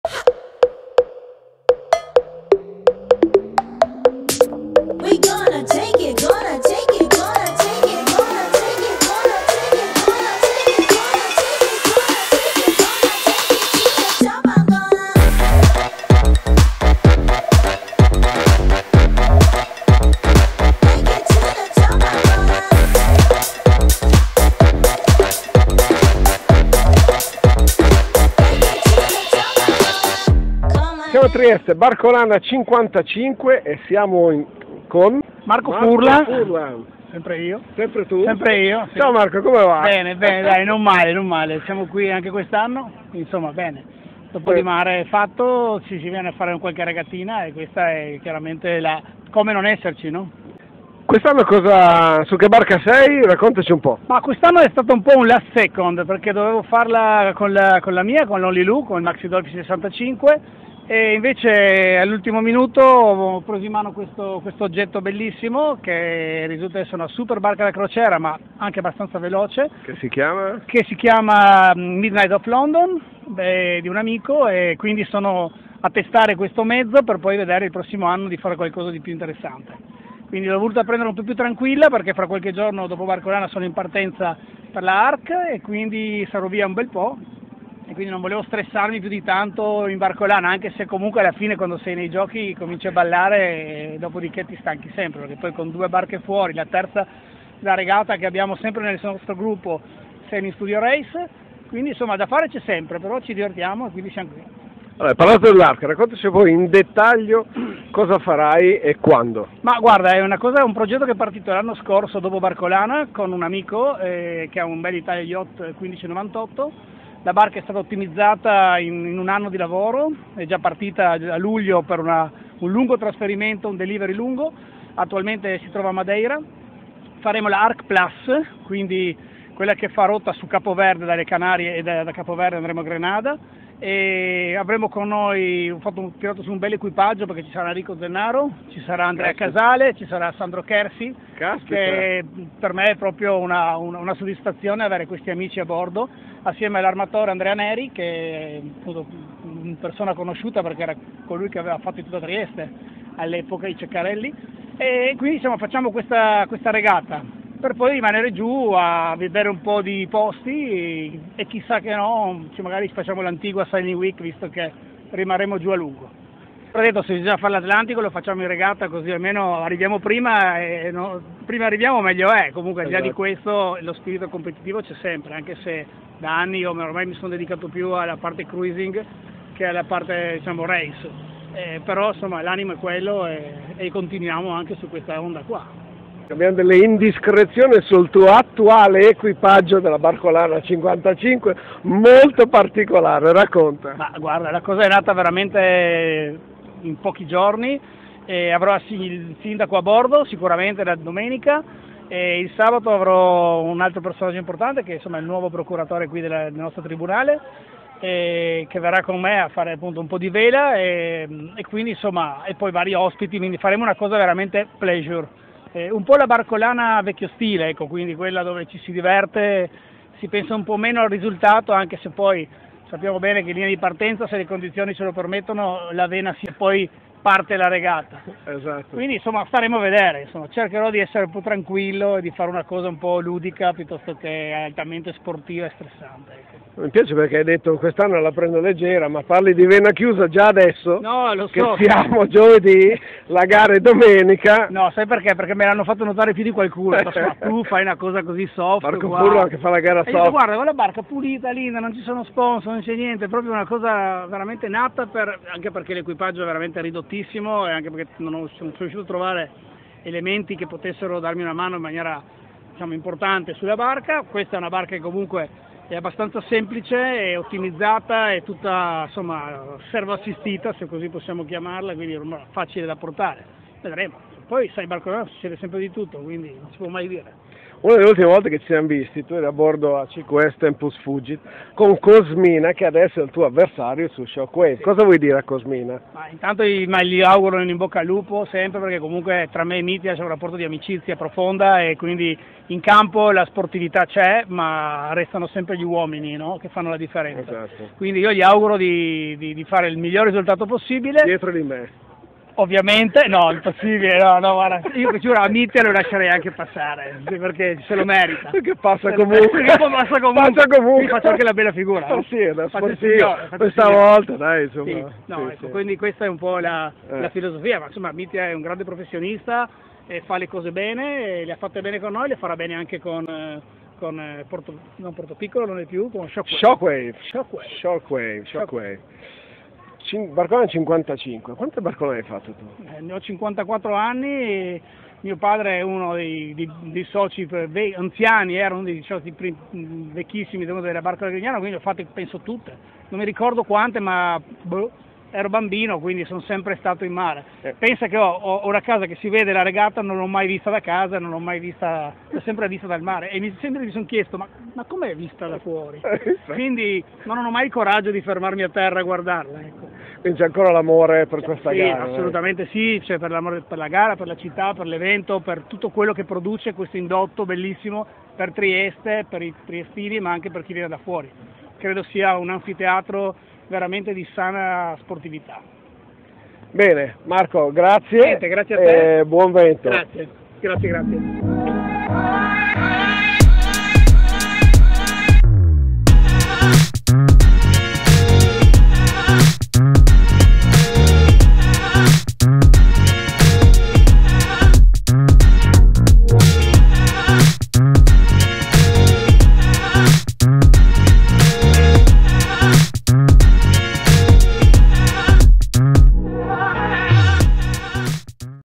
MARCO FURLAN BARCOLANA 2023
Intervista pre  Regata Barcolana 55 -  con Marco Furlan - Skipper di Midnight of London.
Quest'anno nuova sfida per lo skipper Triestino, che al timone della stupenda barca Midnight of London, con un equipaggio  VIP eccezionale a bordo, cercherà di spiazzare i suoi agguerriti avversari ! ! !